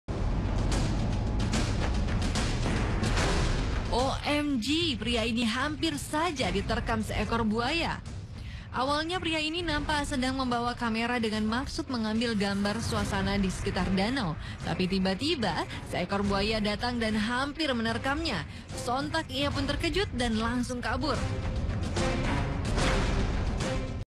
OMG- Di Terkam Buaya Saat Ingin Melihat Danau
'TRAGISS!!Awalnya sih cuman mau liat suasana danau tetapi seekor buaya menerkam dia '